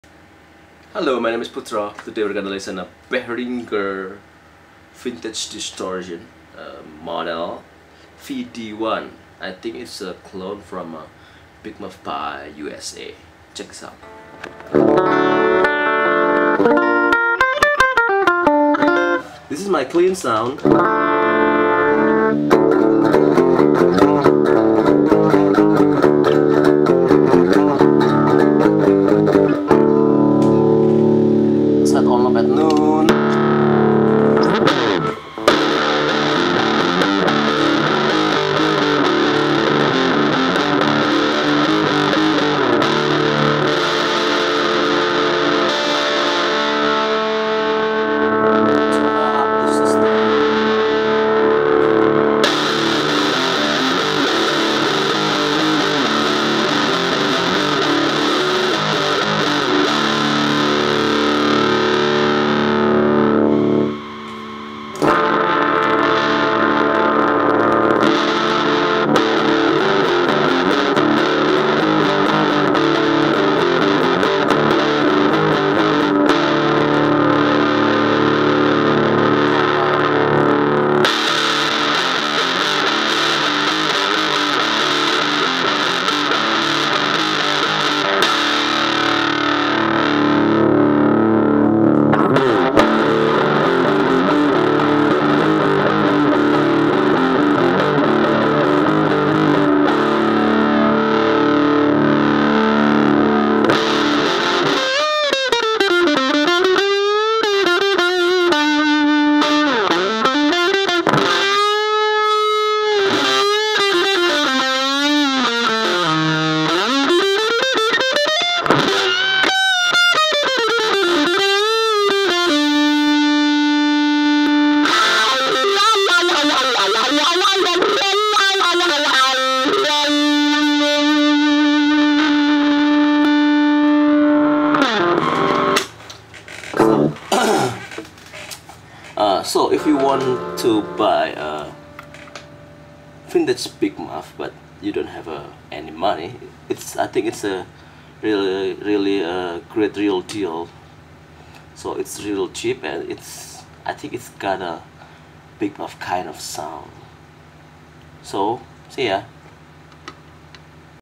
Behringer Vintage Distortion VD-1
Here’s an authentic, classic distortion effect – the amazing VINTAGE DISTORTION VD1 exudes all the warm and emotive tone that has ruled rock for the last 30 years. With its dedicated Tone and Sustain controls, the VD1 will teach your guitar how to scream!